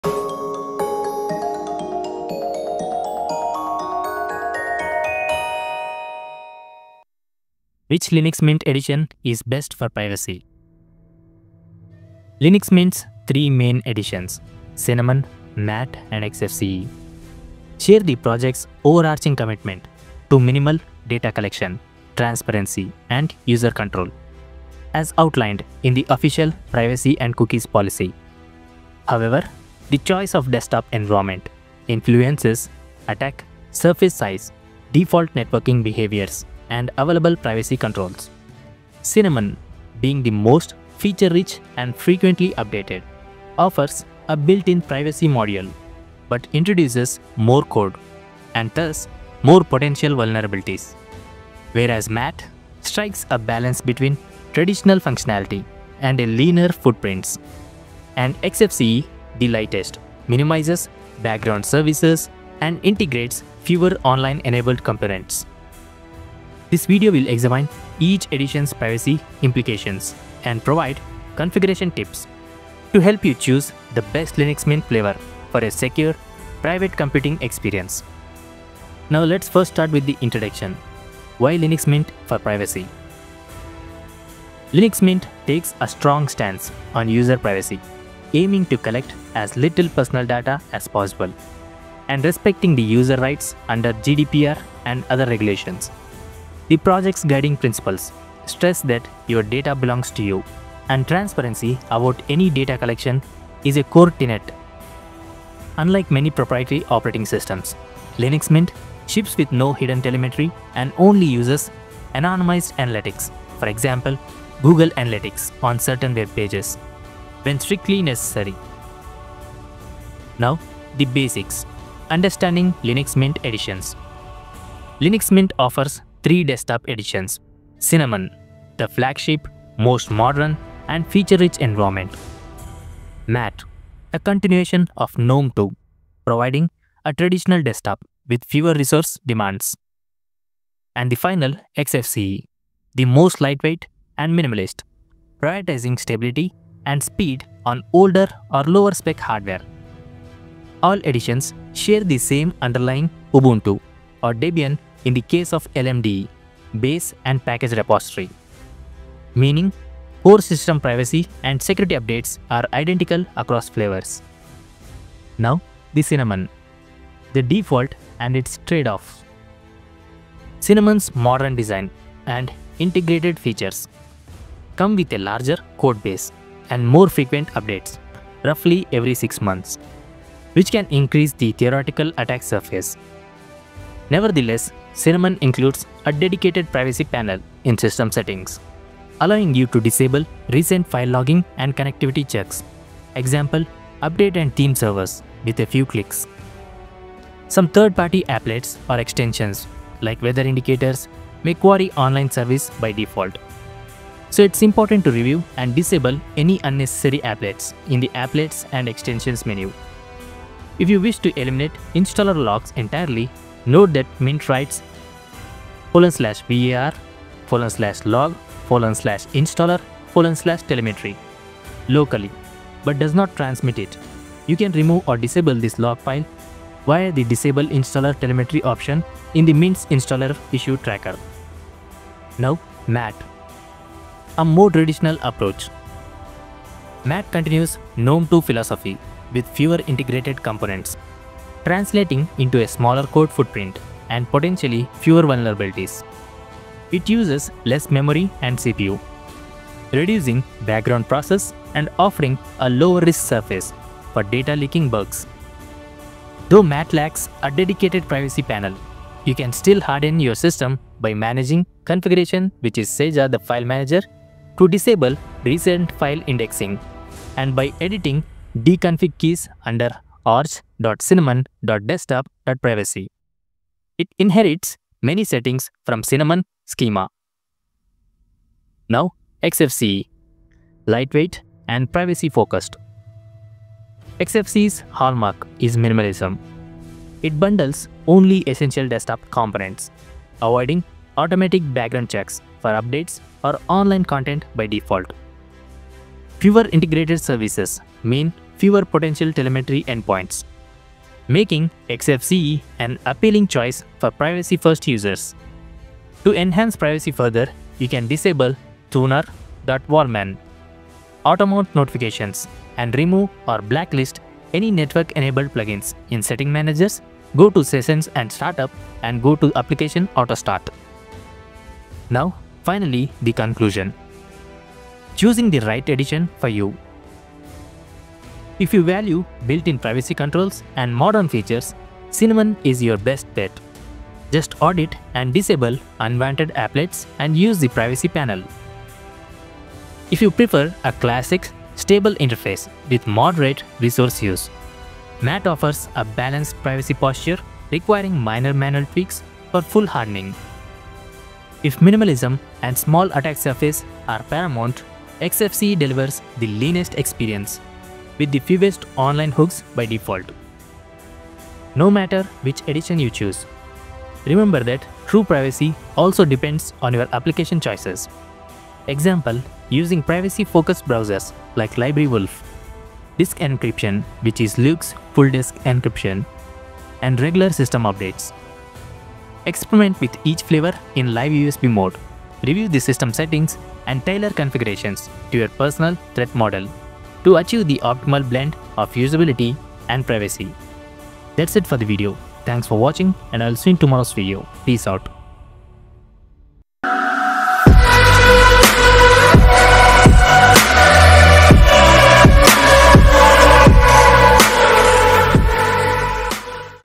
Linux Mint Privacy: Which Edition is Best? (Cinnamon/MATE/Xfce)
Is Linux Mint the right choice for your privacy in 2025? This deep dive explores the privacy implications of Linux Mint's popular editions: Cinnamon, MATE, and Xfce.

Choosing a Linux distribution with privacy in mind is crucial, and Linux Mint offers a great starting point. But does the desktop environment affect your data? We'll break down:

✅ Why Linux Mint is a go-to for privacy-focused users.
✅ Core differences between Cinnamon, MATE, and Xfce.
✅ Potential privacy concerns with Cinnamon's user-friendliness and built-in software – and how to mitigate them!
✅ MATE's traditional, stable approach and its privacy advantages, especially on older hardware.
✅ Xfce's lightweight nature, minimal footprint, and how it can boost your privacy and control.

By the end of this guide, you'll understand the privacy trade-offs of each Linux Mint edition and be able to choose the one that best aligns with your security and anonymity needs. We'll also offer tips on customization and privacy-focused tools for each.

Don't just install Linux Mint; configure it for maximum privacy!

TIMESTAMPS:
0:00 Intro: Why Linux Mint for Privacy?
1:15 Understanding Linux Mint Editions (Cinnamon, MATE, Xfce)
2:30 Cinnamon: Default Edition & Privacy Trade-offs
4:00 MATE: Traditional & Stable for Privacy
5:15 Xfce: Lightweight & Privacy-Focused
6:30 Conclusion: Which Linux Mint Edition for YOUR Privacy?
7:15 Further Customization & Resources